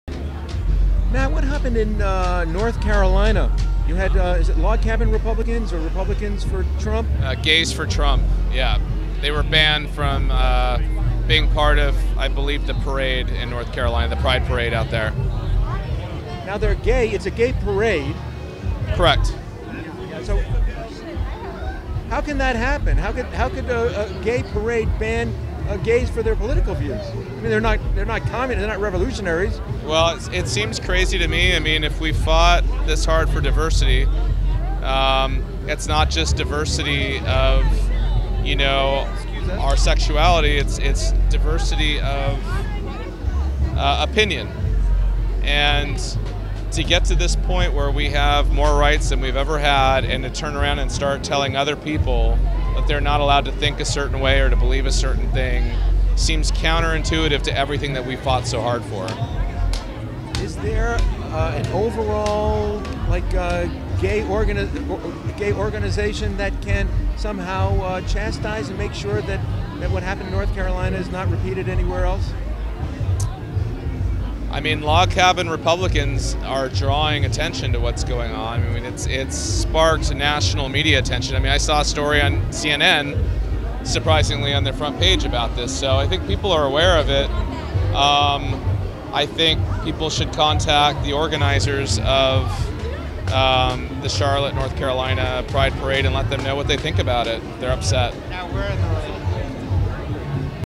Gay Pride Parade banned pro-Trump LGBT group- Matthew Craffey of Log Cabin Repubs reacts
"Charlotte Gay Pride Parade bans Gays for Trump group"
By Jessica Chasmar - The Washington Times - Thursday, June 8, 2017
Members of a gay, pro-Donald Trump group in North Carolina are considering taking legal action after they were prohibited from participating in the upcoming “Charlotte Pride” festival and parade.
A Charlotte Pride spokesperson said the organization reserves the right to deny applications from groups that “do not reflect the mission, vision and values of our organization,” a local Fox affiliate reported.
Brian Talbert, of Gays for Trump, said his group shouldn’t be vilified for supporting the president.
“We wanted to show that we weren’t the racist, bigot, misogynist whatevers,” he told Fox. “We wanted to show that we are Americans, love our country and our president. We wanted to be there to celebrate gay pride.
“For a group of people to claim to want tolerance and acceptance … for them to sit back and judge me for exercising my right as an American to choose my leader without judgment — it’s hypocritical,” he said.
Charlotte Pride’s statement invited “all” people who “share our values” to attend the Charlotte Pride Festival and Parade, Aug. 26-27.
“Charlotte Pride envisions a world in which LGBTQ people are affirmed, respected and included in the full social and civic life of their local communities, free from fear of any discrimination, rejection, and prejudice,” the organization said.
Mr. Talbert said he plans to file a lawsuit against Charlotte Pride for discrimination.
“I don’t judge them for how they vote,” Mr. Talbert told Fox. “I believe men and women died to give us that right to choose our own leader. They don’t extend the same courtesy to me being a gay Republican.”